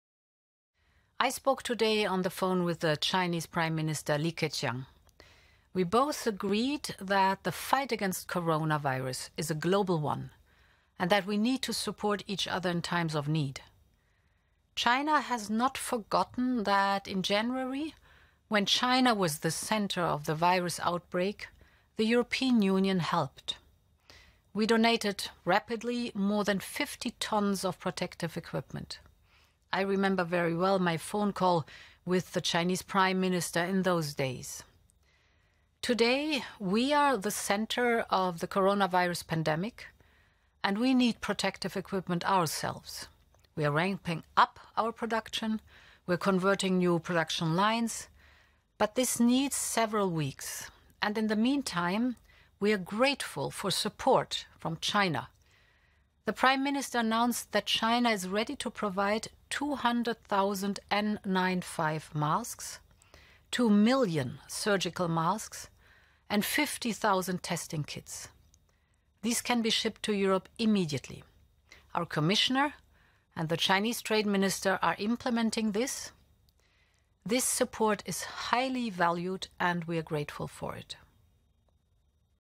Videoboodschap van Ursula von der Leyen over Chinese hulp aan de EU voor de bestrijding van COVID-19
Commissievoorzitter Ursula von der Leyen bedankt de Chinese premier voor de toegezegde levering van beschermend materiaal om COVID19-besmettingen tegen te gaan.